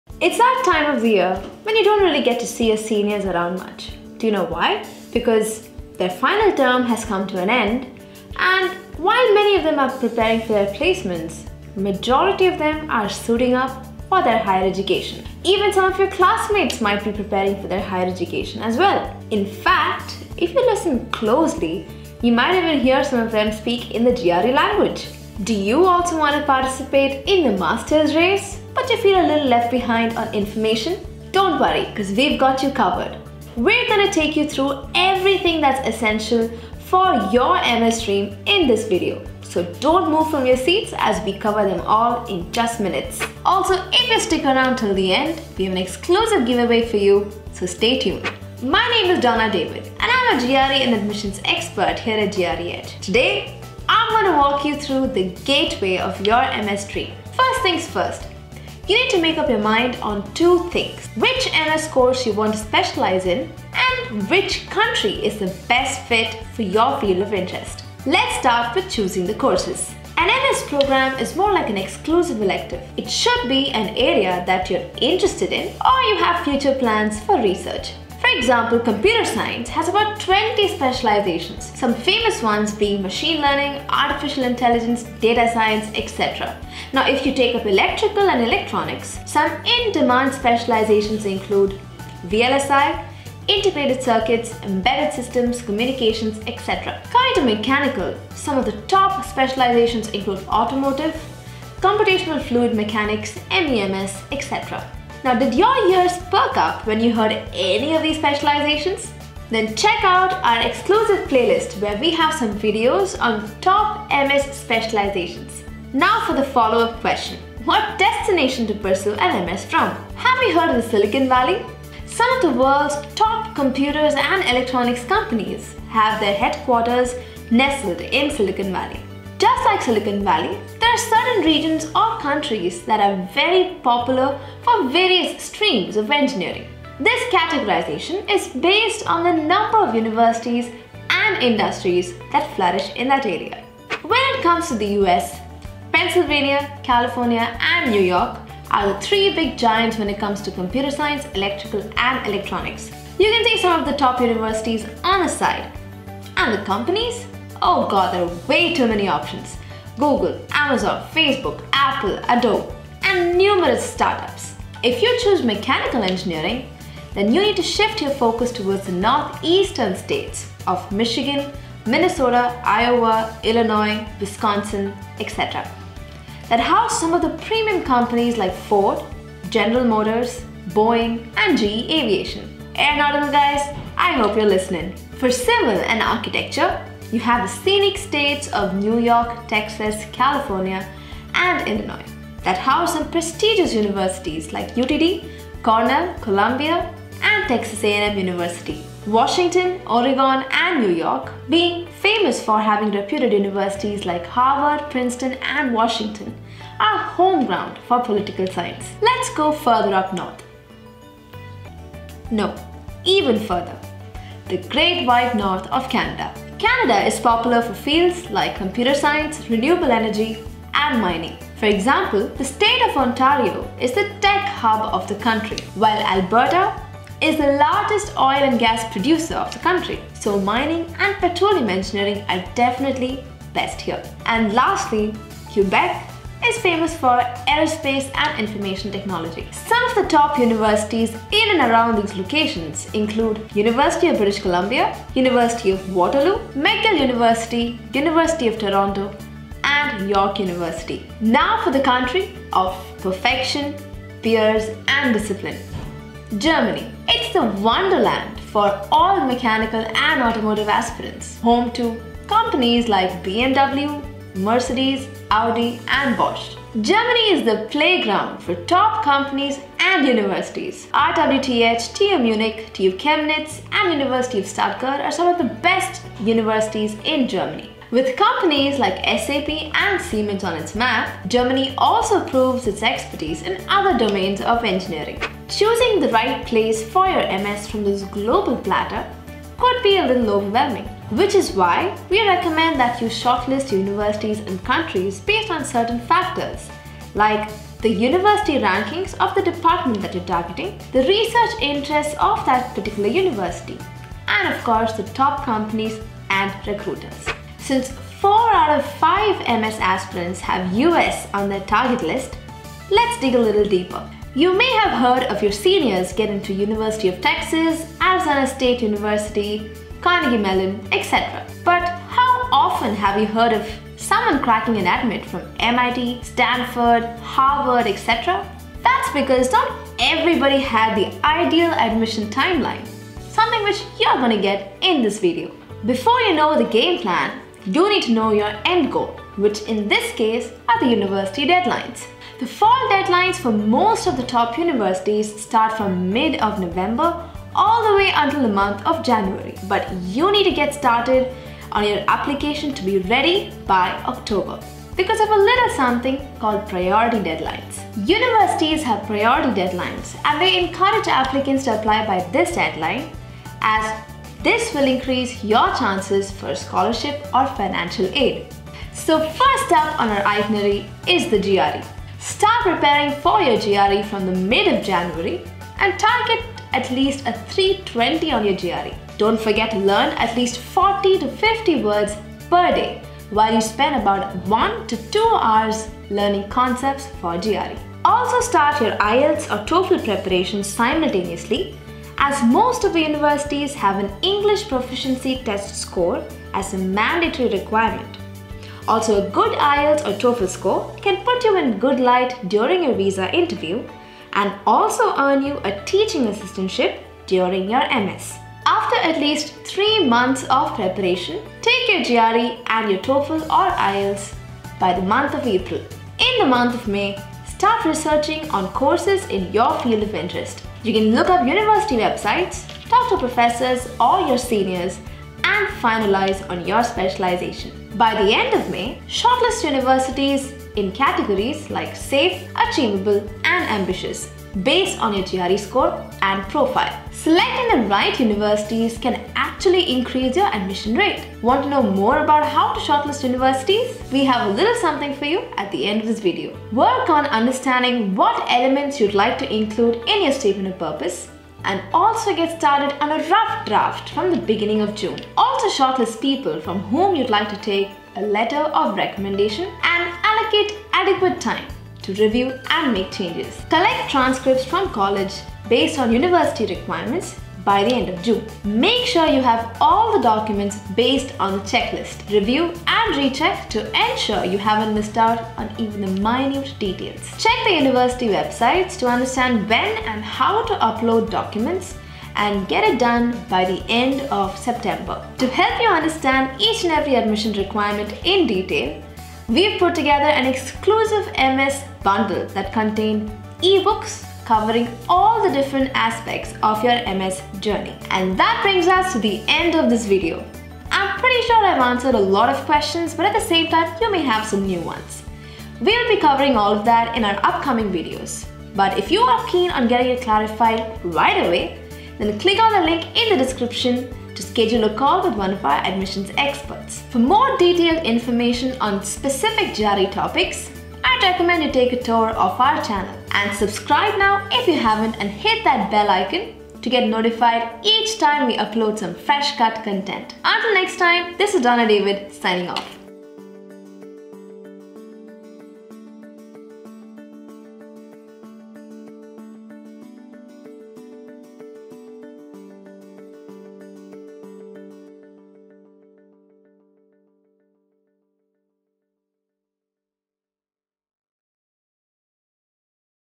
MS in US: FAQ | How to choose a master specialisation?
Decoding all your admission related doubts.
We will be covering -
# MS Specialisation
# Selecting Country/ Universities basis course
# Admission Timeline & Process
An MS specialization is more like an exclusive elective.
It should be an area that you're interested in and want to pursue research.

Heard of Silicon Valley for MS in US?
Top Computer and Electronics companies of the world have their headquarters nestled in here.

When it comes to the MS in US, Pennsylvania, California and New York are the landmarks for Computer, Electrical, and Electronics. You can see top universities in these regions listed on the side. And the companies?

Google, Amazon, Facebook, Cisco, Paypal, Apple, Adobe, etc. and there are also not short on startups.

If you are a Mechanical engineer then you need to paint the northeastern states of Michigan, Minnesota, Iowa, Wisconsin, and Illinois, red.
The companies after MS in US?
Ford Motors, General Motors, Google, Boeing and GE aviation. Ahem, Aeronautical guys, are you listening.

For Civil and architecture, Iowa and the scenic states of New York, Texas, California and Illinois with the esteemed universities of UTD, Cornell, Columbia, and TAMU for MS in US.

You must have heard stories of your seniors getting into UTD, CMU, ASU and the others for their MS in US.
But have you heard any of them cracking an admit from the likes of MIT, Stanford, and Harvard? No right?
Because they did not have something called the Ideal Admission Timeline, something that you are going to get for MS in US.
Before you know the game plan you need to know your end goal, which here are the University deadlines for MS in US.
The Fall deadline of top universities commence from mid of November and go on till the Month of January. But you should be ready by October because of the little something called Priority deadlines for MS in US.
Universities have priority deadlines and encourage the applicants to apply by this deadline as this will increase your chance for a scholarship and/or financial aid.
SO
First up on our itinerary is the GRE.

Start your GRE preparation from the mid of January. Target a score of 320 and spend at least 1-2 hours/ day and also learn about 40-50 GRE words.

Also start your IELTS preparation simultaneously as most of the universities have an English proficiency test score as a mandatory requirement. A good IELTS score can put you in good light during your VISA interview and also earn you a Teaching Assistantship during your MS in US.

After at least 3 months of preparation, take your GRE and IELTS in the month of April.

Start researching the courses in your field of interest. You can search the universities’ websites, talk to professors or your seniors and decide to finalize on your specialization for MS in US.

Shortlist universities as Safe, Achievable and Ambitious based on your GRE score and profile. Selecting the right set of universities can improve your admission rate.

Work on understanding the elements needed for your SOP and start working on a rough draft at the beginning of June. Also, shortlist people from whom you require LOR’s and allocate adequate time to review and make changes

Collect transcripts from college based on the university requirements.

Make sure you have all the documents according to the checklist. Review and recheck to ensure you haven’t missed out on even the minute details.

Check the university websites to understand where and how to submit your documents. Upload them by the end of September.

To help you know about each section of your admission requirement we have an exclusive MS in US eBook bundle that contains an eBook covering all the aspects of your MS in US linked above!

I’m pretty sure that most of your queries are answered but at the same time, you have new ones.  We will be addressing all of it in the upcoming video but if you are keen are getting clarity right away, talk to an admission expert today regarding your MS in US dreams!